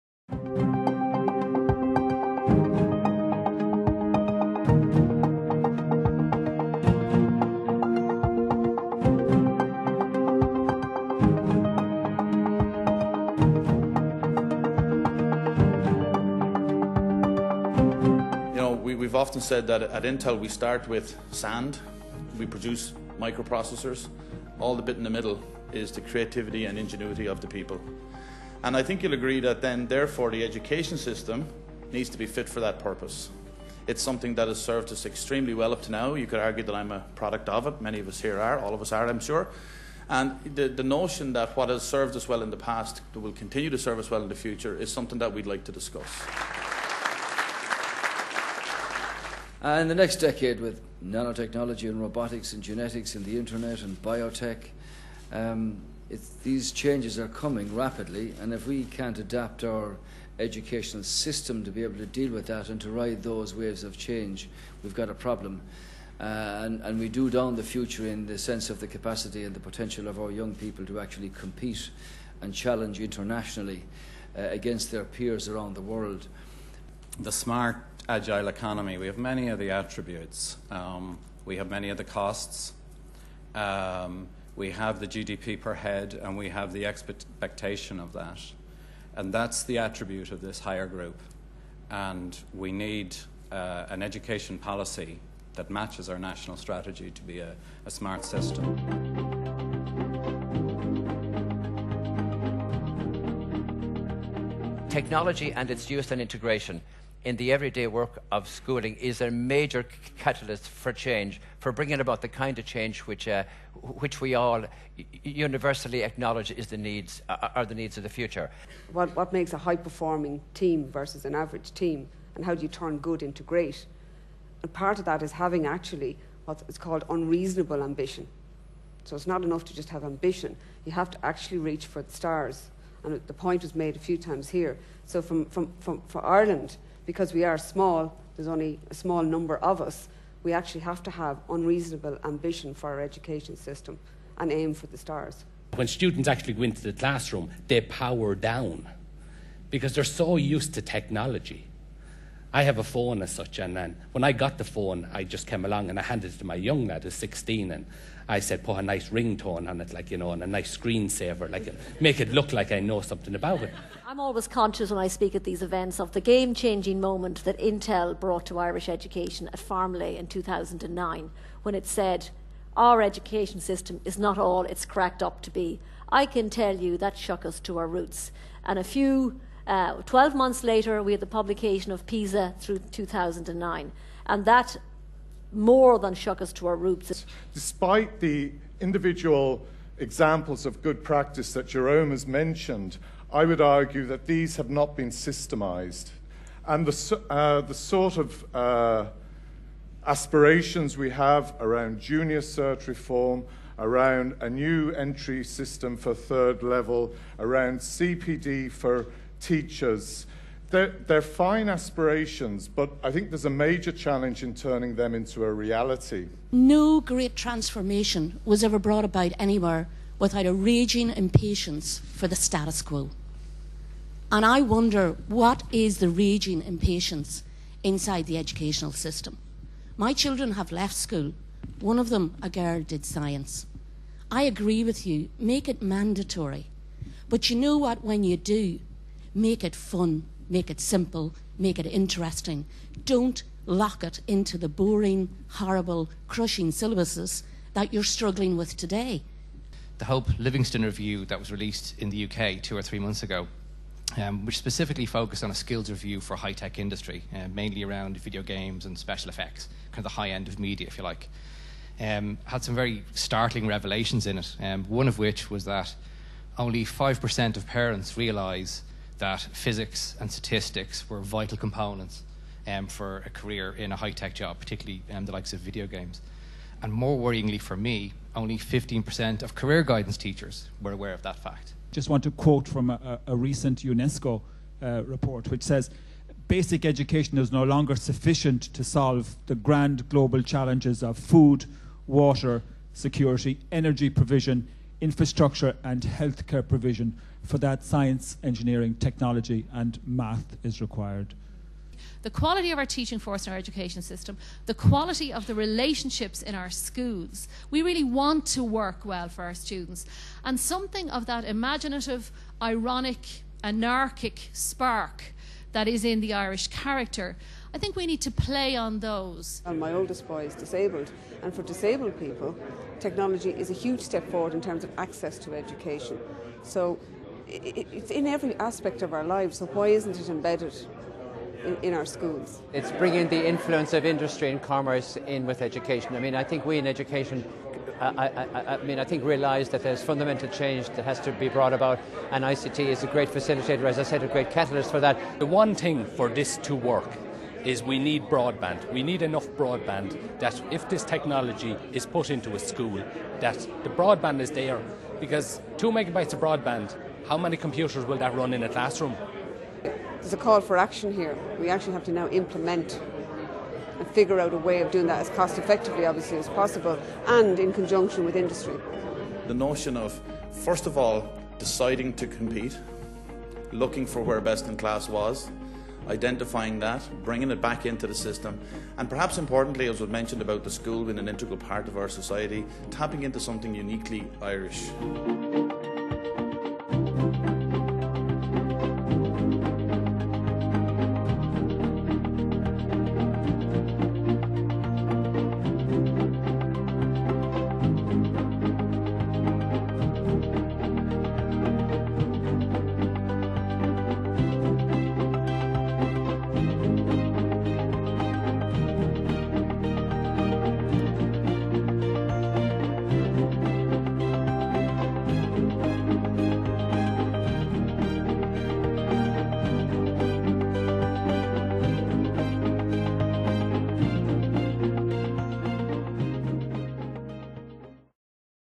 Transforming Education in Ireland - Intel Open Forum event
Intel Ireland recently hosted an Education Transformation Open Forum in the Science Gallery, Trinity College, Dublin. The event brought together stakeholders in the education process and provided a platform to gather diverse perspectives regarding our education system and suggest possible avenues to ensure that it is fit for purpose in a 21st century knowledge based economy .